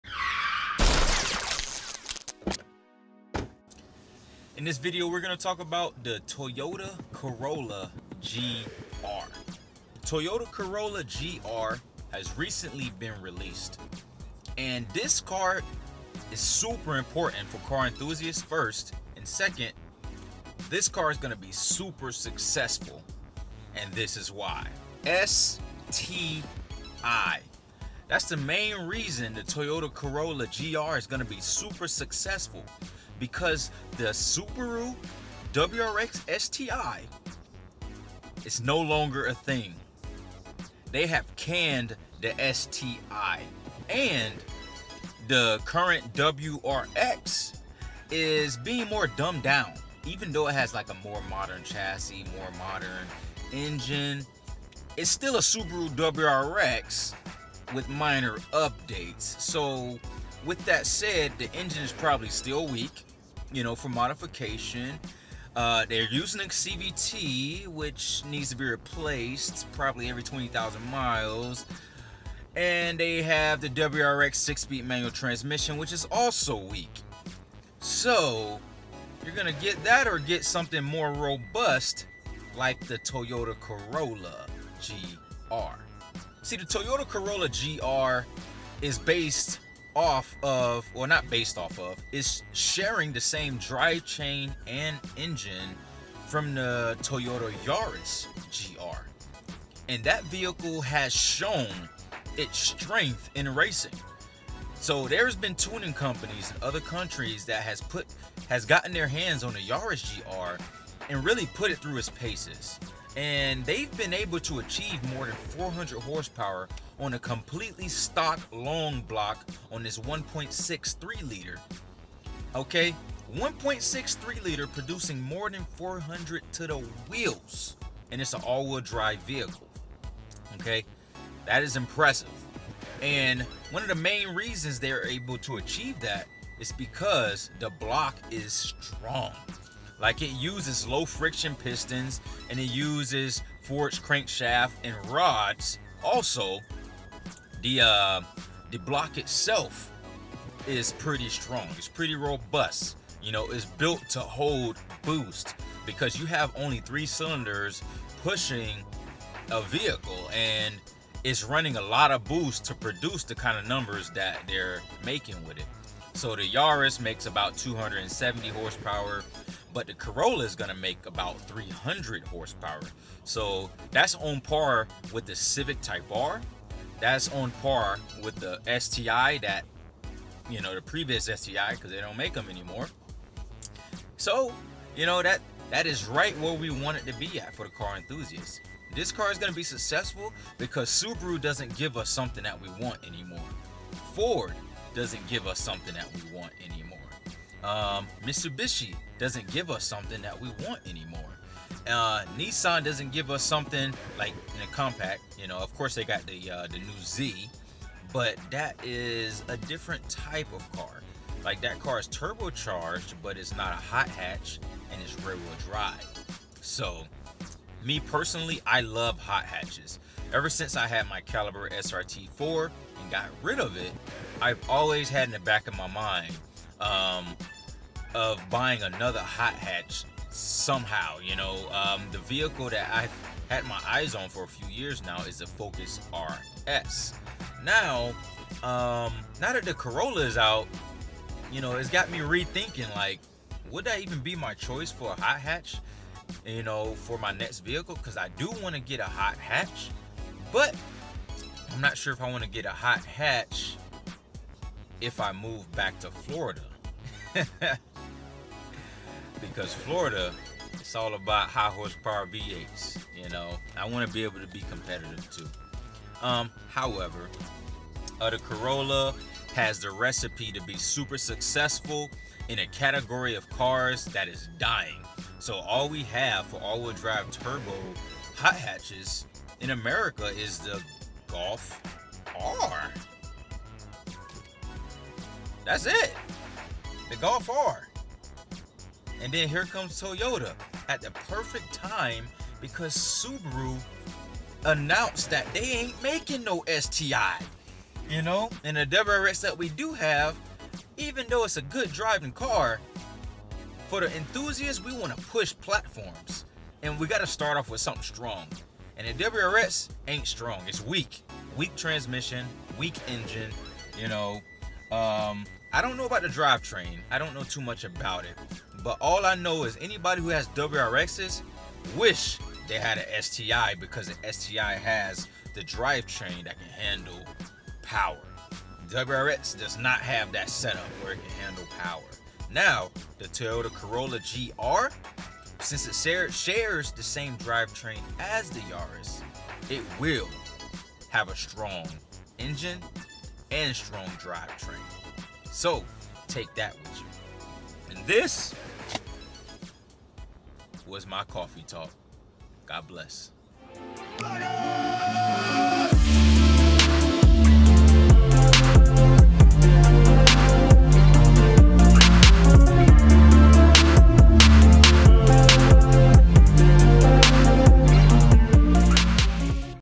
The GR Corolla was announced at the perfect time: Coffee Talk
Toyota is back!!! In a dying breed of cars like the Mitsubishi Lancer evolution and Subaru wrx sti at there deathbed, Toyota has come to the rescue.